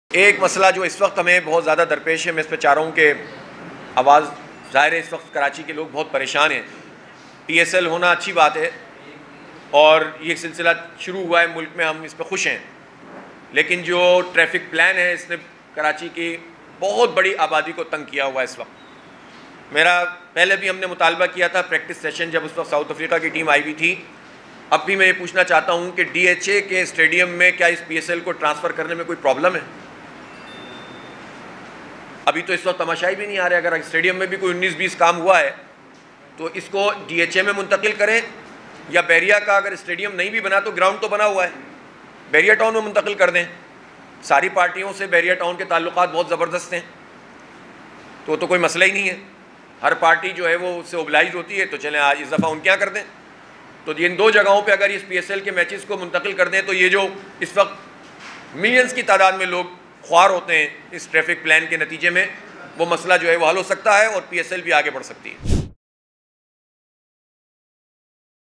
پی ایس ایل کو ڈی ایچ اے یا بحریہ ٹاؤن کے گراؤنڈ میں کیوں منتقل نہیں کیا جاتا؟ حافظ نعیم الرحمن
کراچی میں شدید ٹریفک جام سے پریشان عوام کو ریلیف دینے کے لئے پی ایس ایل کو  ڈی ایچ اے اسٹیڈیم یا بحریہ ٹاؤن کے گراؤنڈ میں کیوں منتقل نہیں کیا جاتا ۔ حافظ نعیم الرحمن